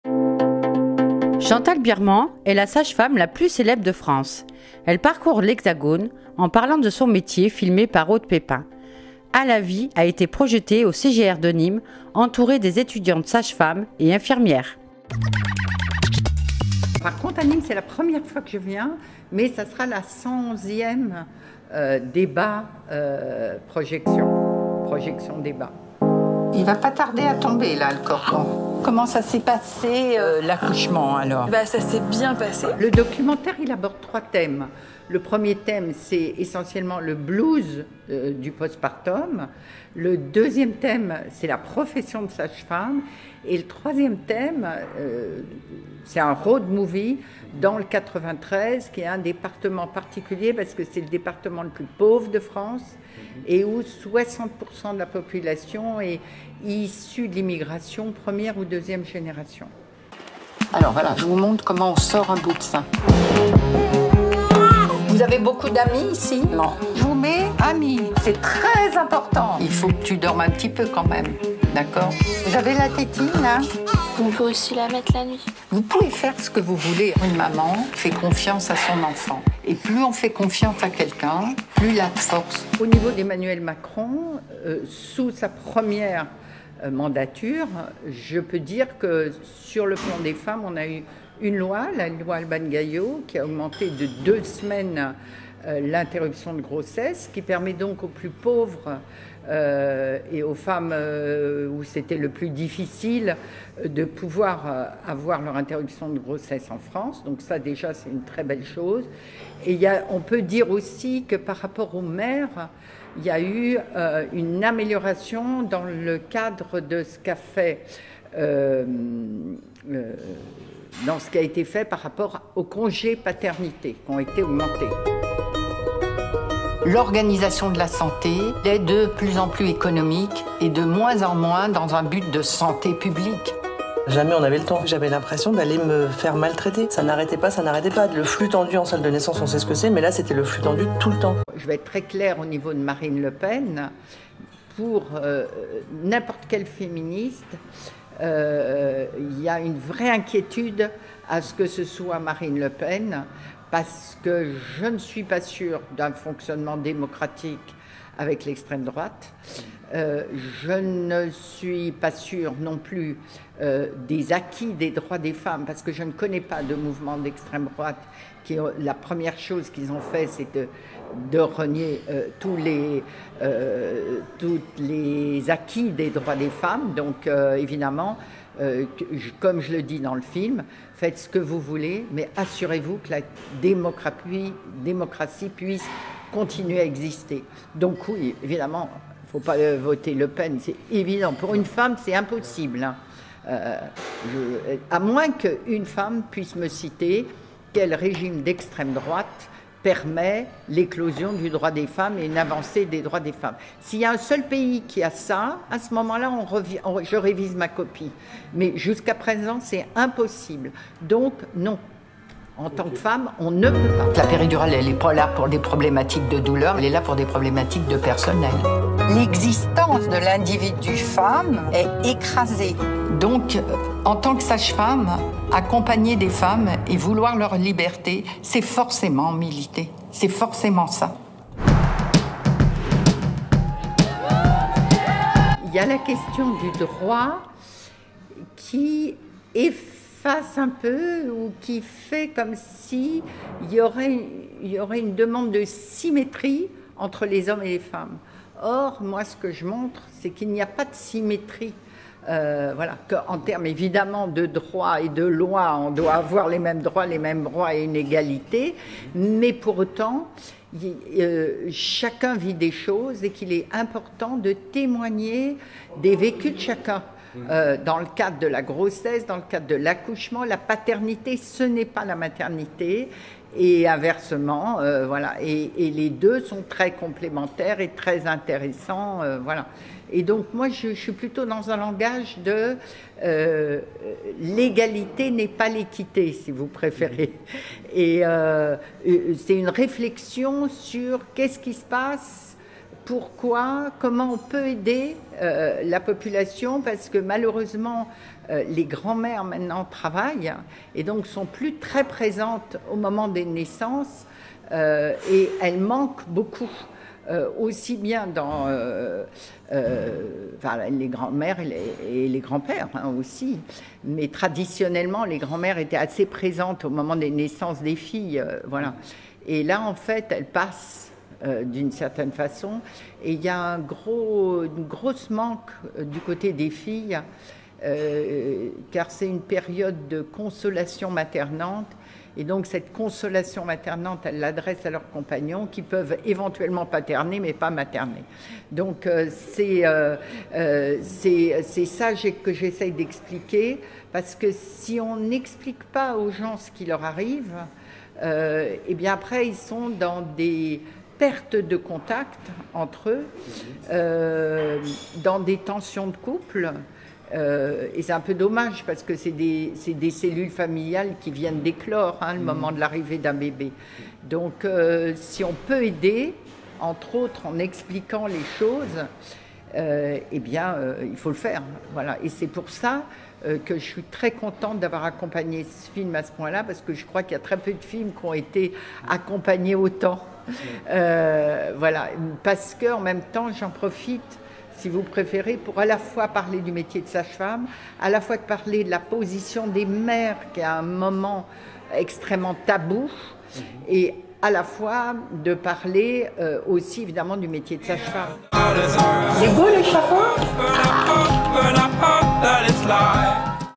À LA VIE : Chantal Birman sage-femme appelle à ne pas voter Marine Le Pen (ITW CGR Nîmes)
La 1ère sage-femme de France parle du danger de voter les extrêmes, pour une femme qui élever son enfant !